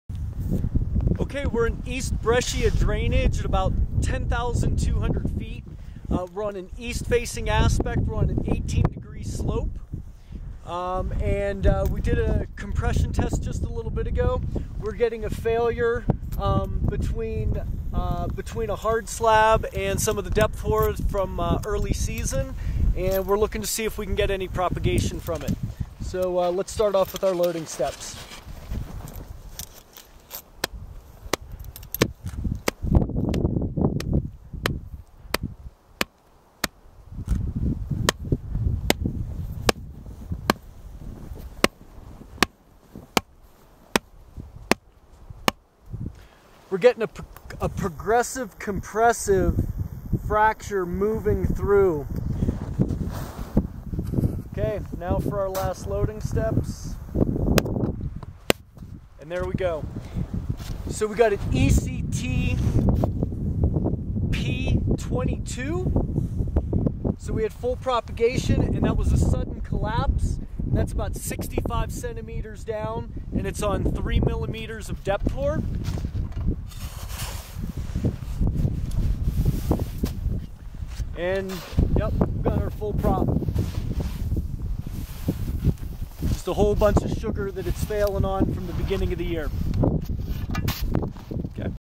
Snowpack Test / ECT / 1/19/14
Performing an Extended Column Test in East Breccia Drainage on January 19, 2014. Results: ECTP 22